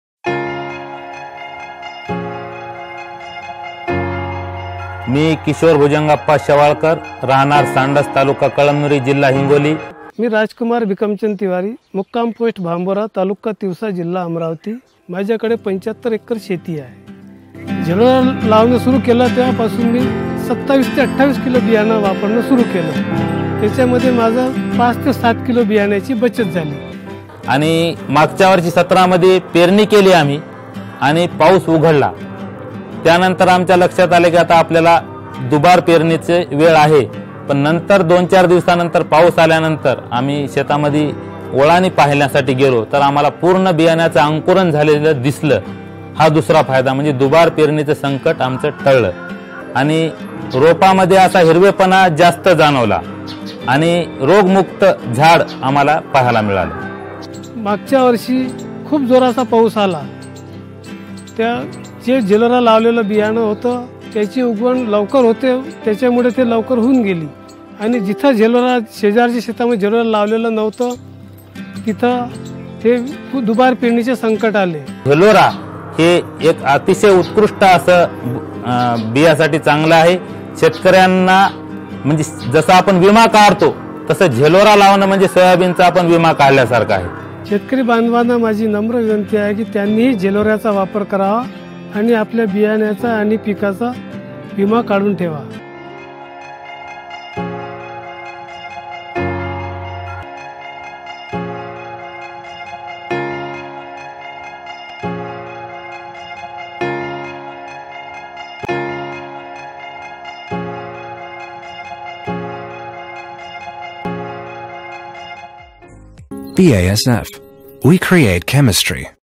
बीएएसएफ झेलोरा - सोयाबीन बीजप्रक्रियामुळे मिळणाऱ्या फायदयाबद्दल शेतकऱ्यांचे अनुभव
प्रिय शेतकरी बंधुनो,
बीएएसएफ “झेलोरा- एक योग्य सुरुवात” हे थायोफॉनेट मिथाइल सहीत AgCelenceचे फायदे असलेलं बीजप्रक्रिया करण्यासाठी एक अनोखे एफ यस फॉर्म्युलेशन असणारे उत्तम दर्जाचे बुरशीनाशकं आहे  जेंव्हा मिळेल झेलोराची ची साथ, सोयाबीन उत्पादन वृद्धीची होईल योग्य सुरुवात. झेलोरा हे सोयाबीन मध्ये लवकर आणि मोठ्या प्रमाणात अंकुरण , कोवळ्या रोपावरील रोगांवर नियंत्रण आणि अगसलेन्स हे फायदे देते.